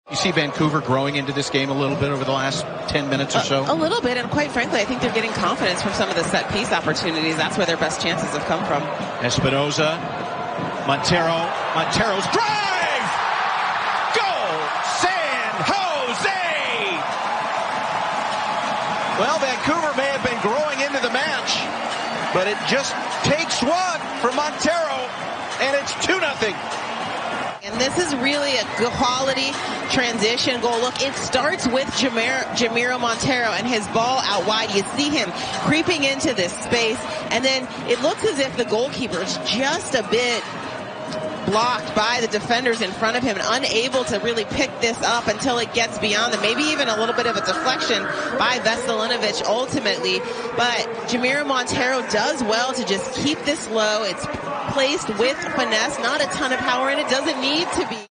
GOAL: JAMIRO MONTEIRO SLIDES ONE INTO THE BACK OF THE NET!!
Jamiro Monteiro scores for the Quakes. Midfielder Jamiro Monteiro scored his fourth goal of the season and 13th of his MLS career. He has now tied his single-season career high in goals (2019).